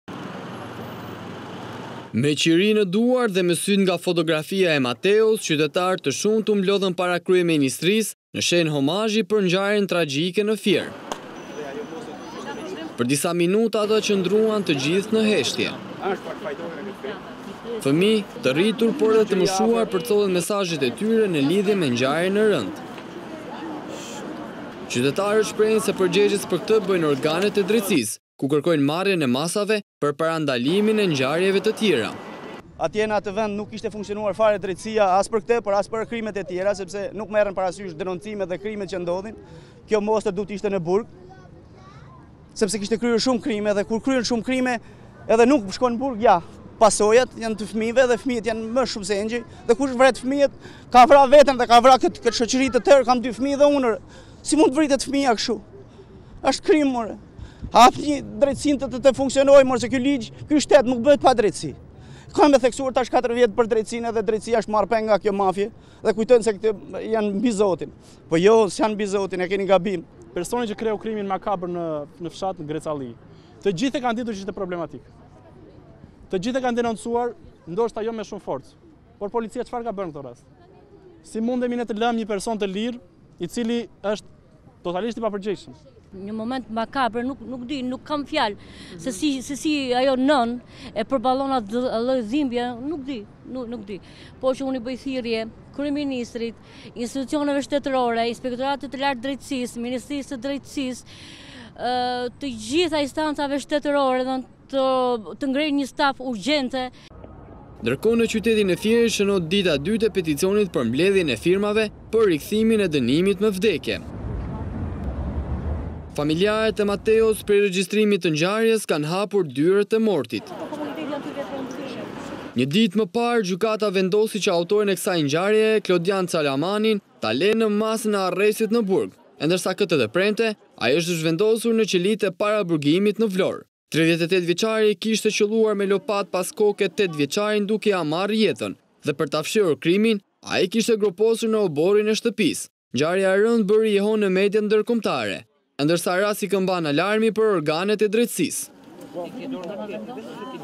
"Mos më vrit, nuk dua të bëhem një engjëll" homazhe para kryeministrisë për vogëlushin Mateo
"Mos më vrit, unë nuk dua të bëhem një engjëll," shkruante pankarta që mbante në duar një fëmijë.

Me qiri në duar dhe me sytë nga fotografia e Mateos qytetarë të shumtë u mblodhën para kryeministrisë në shenjë homazhi për ngjarjen tragjike në Fier.

Për disa minuta ata qëndruan të gjithë në heshtje

Fëmijë, të rritur, por edhe të moshuar përcollën mesazhet e tyre në lidhje me ngjarjen e rëndë.

Qytetarët shprehen se përgjegjës për këtë bëjnë organet e drejtësisë, ku kërkojnë marrjen e masave për parandalimin e ngjarjeve të tjera.

"Atje në atë vend nuk kishte funksionuar fare drejtësia, as për krimet e tjera sepse nuk merren parasysh denoncimet për krimet që ndodhin. Kjo monstër duhet të ishte në burg sepse kishte kryer shumë krime dhe kur nuk shkon në burg ja pasojat. Kush vret fëmijët ka vrarë veten dhe këtë shoqëri. Si mund të vritet fëmija kështu? Ky shtet nuk bëhet me drejtësi, drejtësia është marrë peng nga kjo mafie."

"Personi që krye krimin makabër në Greçalli, të gjithë e kanë ditur që ishte problematik, të gjithë e kanë denoncuar, por polcia çfarë ka bërë në këto raste?"

"Një moment makabër, nuk kam fjalë se si ajo nënë e përballon atë dhimbje. I bëj thirrje të gjitha instancave shtetërore."